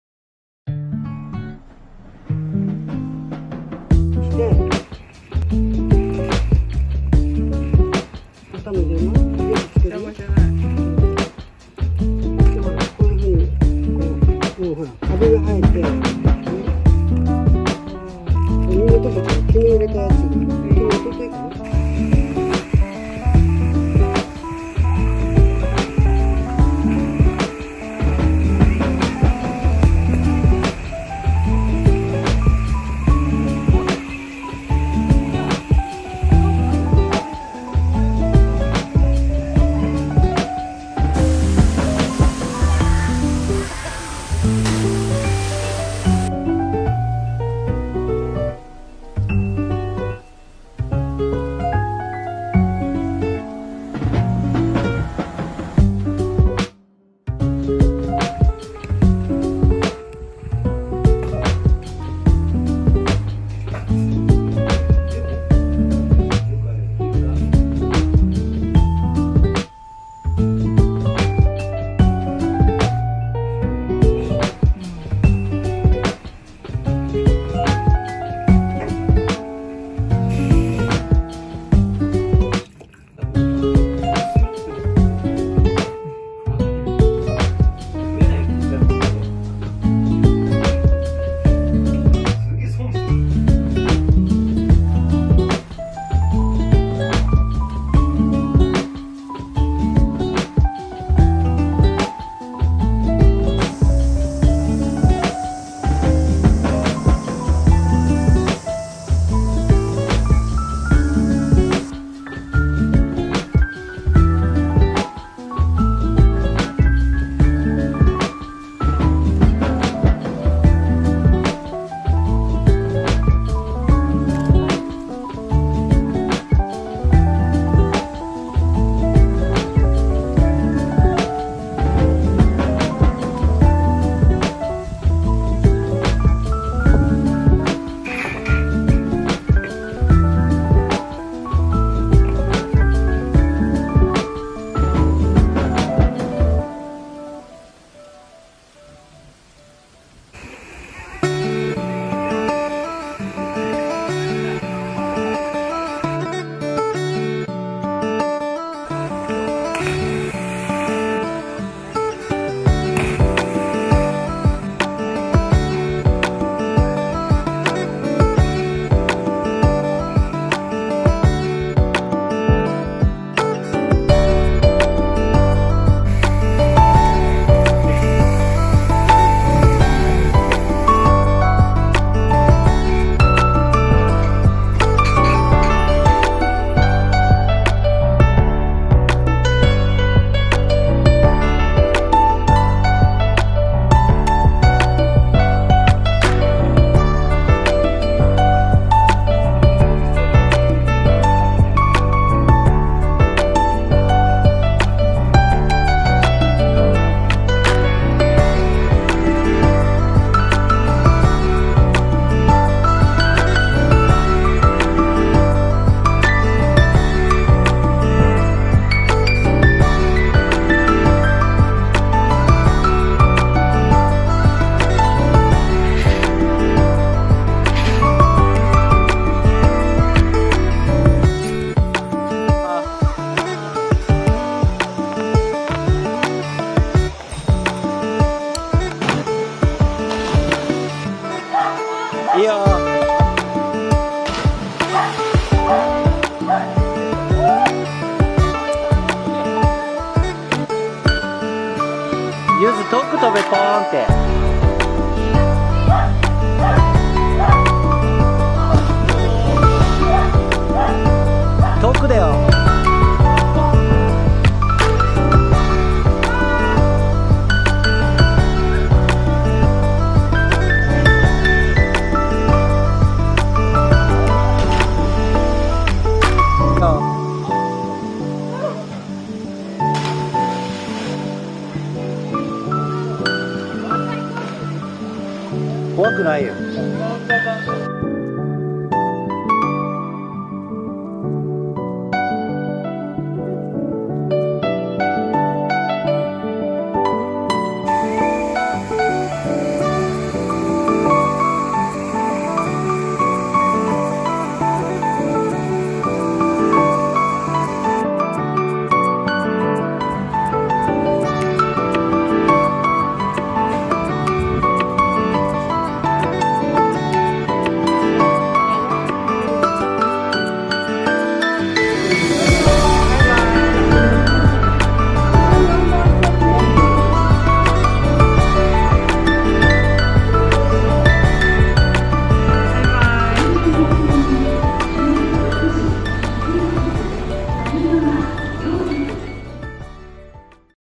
Summer Days Spent in Minamiuonuma, Niigata, Japan | English CC available
I visited my grandparents, aunt, uncle, and cousins who live in Minamiuonuma, Niigata, Japan.
It has been three years since I visited them, so it was such a fun summer vacation.

Minamiuonuma is such a beautiful city that has abundant nature such as mountains and rivers. It's famous for good rice and sake.